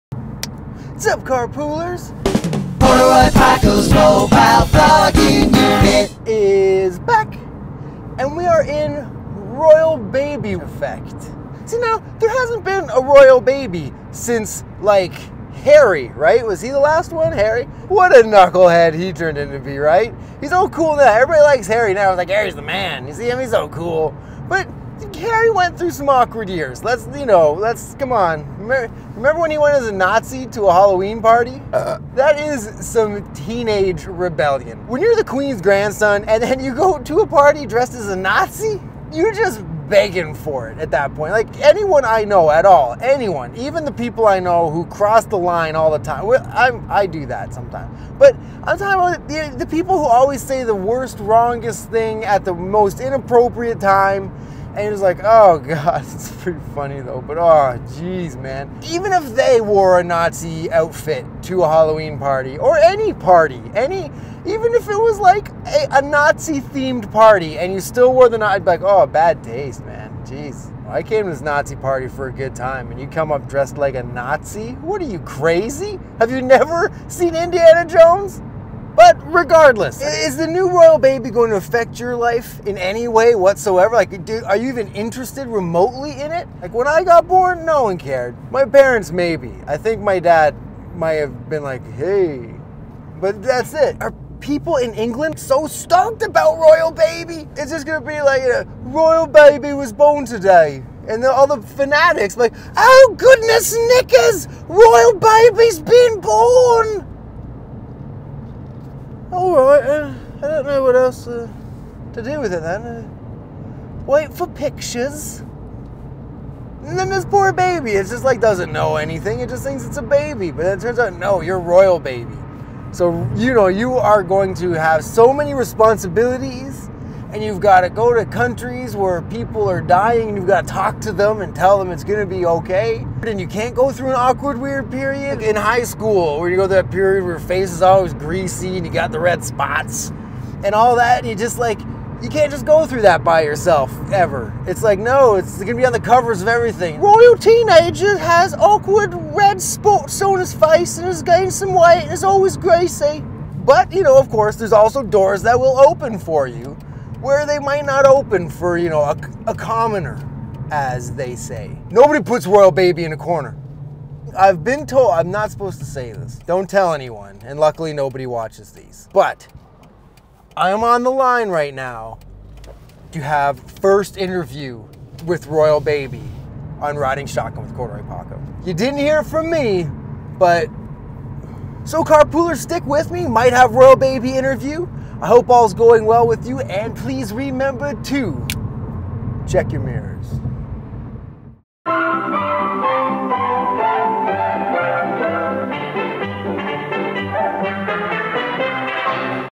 #63 - Royal Baby - Corduroy Paco's Mobile Vlogging Unit
Hey Carpoolers...It's a joyous time to be alive, isn't it?  A baby has been born!  Not just any run of the mill baby - but a Royal Baby!  Wow!  Wait...does anyone REALLY care?  Not that people DON'T care but...I don't even know what any of this actually means.  And I prove it in the latest edition of the Mobile Vlogging Unit!